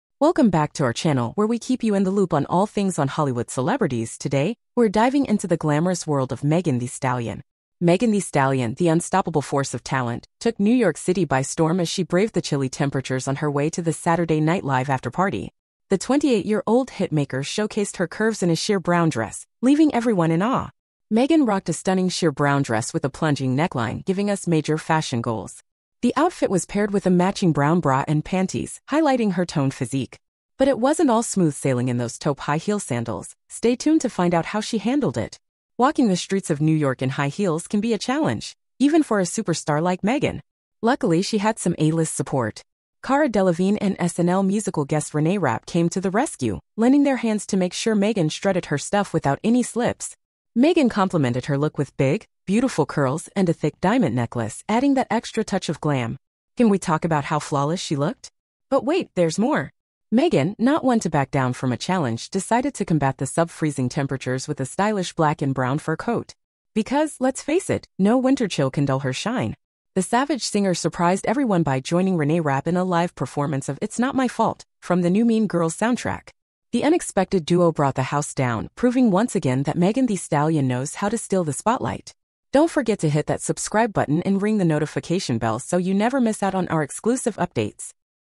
Megan Thee Stallion Stunning Arrival at SNL afterparty with help from Renee Rapp & Cara Delevingne
Megan Thee Stallion braved the cold New York temperatures to showcase her toned body on her way to the Saturday Night Live after party. with help from Renee Rapp and Cara Delevingne.